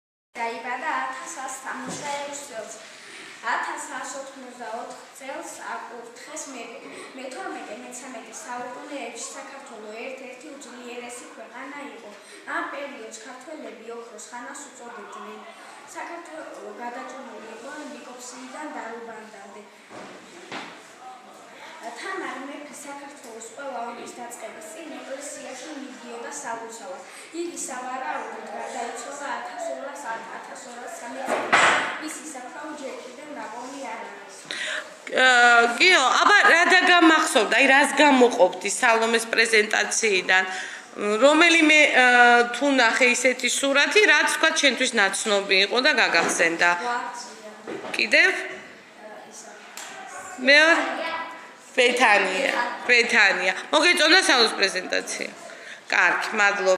გიორგი სურგულაძე. მე-5 კლასი .
ჩვენი საქართველო . ნანახის გააზრება და ჩანაწერის გაკეთება
თამარ მეფე